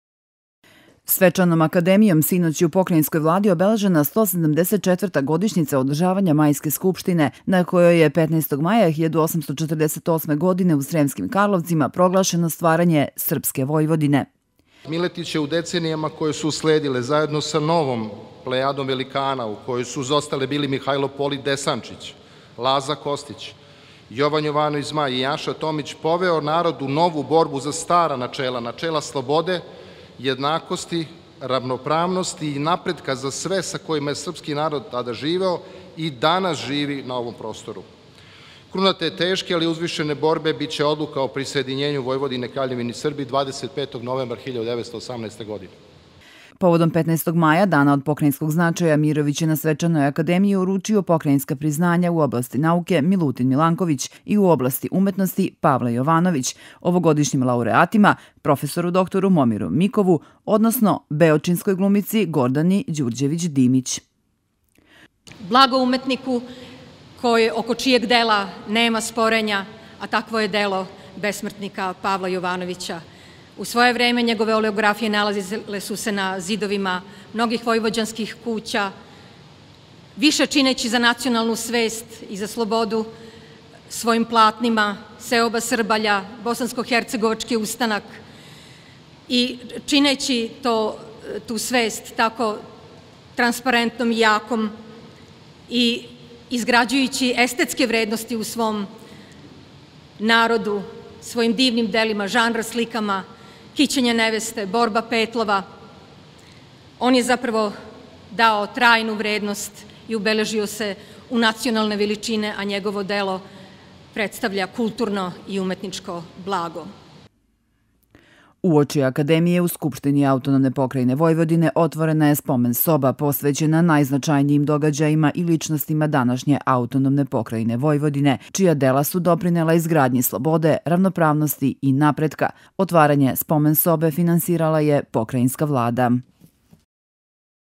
Pokrajina 17052022
PRILOG POKRAJINA
Свечаном академијом синоћ је у Покрајинској влади обележена 174. годишњица одржавања Мајске скупштине, на којој је 15. маја 1848. године у Сремским Карловцима проглашено стварање Српске Војводине.
Sync Igor Mirović, predsednik Pokrajinske vlade
1,23 Miletić je u decenijama koje su usledile
1,53  25. novembra 1918. godine
OF: Поводом 15. маја, дана од покрајинског значаја, Мировић је на свечаној академији уручио покрајинска признања у области науке „Милутин Миланковић“ и у области уметности „Павле Јовановић“ овогодишњим лауреатима – проф. др Момиру Микову, односно beočinskoj glumici Гордани Ђурђевић Димић.
SYNC Gordana Đurđević Dimić, glimica SNP-a’
1,12 delo besmrtnika pavla jovanovića, njegove oleografije nalazile su se
1,59 kulturno I umetničko blago
Уочи академије, у Скупштини АП Војводине, отворена је Спомен-соба посвећена најзначајнијим догађајима и личностима данашње АП Војводине, чија дела су допринела изградњи слободе, равноправности и напретка као темељних принципа и стубова наше отаџбине. Отварање Спомен-собе финансирала је Покрајинска влада.